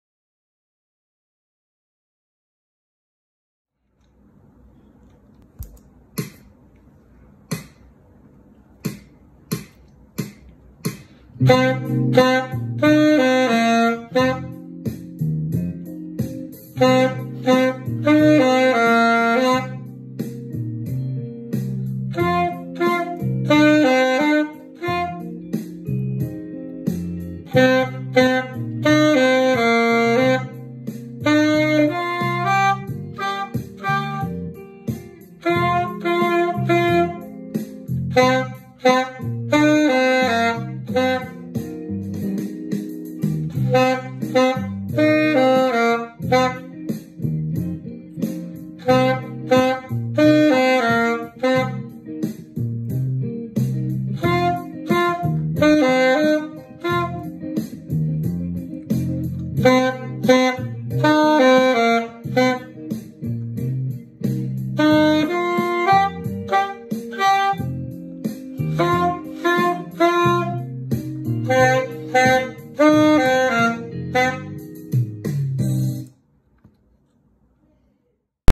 Jayden playing the blues
Here's Jayden, who was only 7 when he recorded the blues!
He was one month shy of his 8th birthday.
He is a really motivated student of mine who loves to play the saxophone.